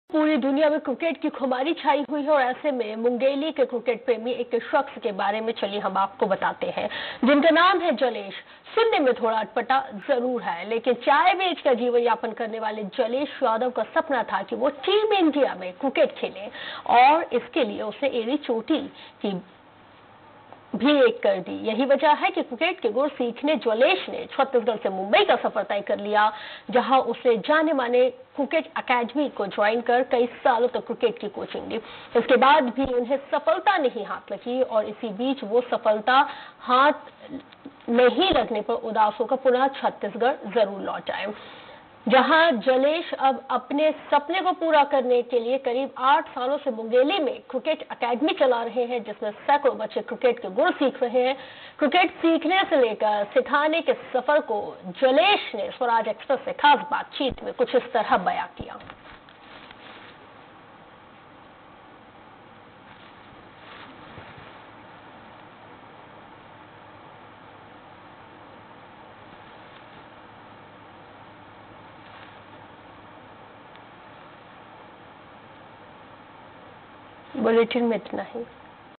8 सालों से मुंगेली में क्रिकेट अकेडमी चला रहा ये क्रिकेट प्रेमी, बच्चों को सिखा रहे क्रिकेट के गुण
8 सालों से मुंगेली में क्रिकेट अकेडमी चला रहा ये क्रिकेट प्रेमी, बच्चों को सिखा रहे क्रिकेट के गुण